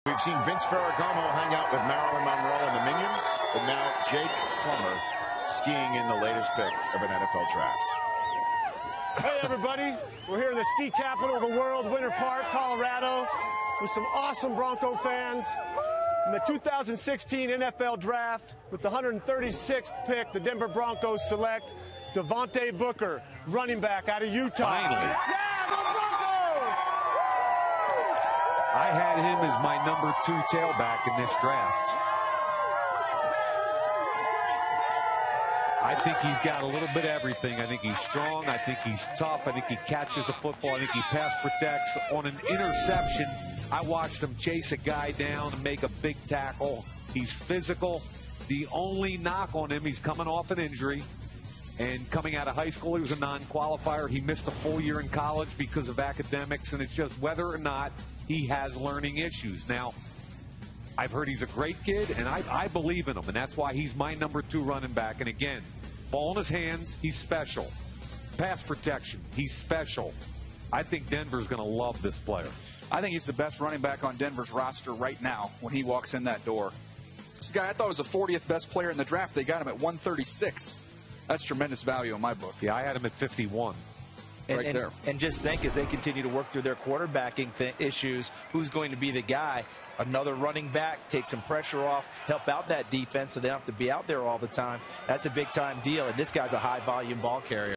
Devontae Booker (RB) | Pick 136: Denver Broncos | 2016 NFL Draft
In the 4th round, the Denver Broncos draft Utah RB Devontae Booker with the #136 pick in the 2016 NFL Draft.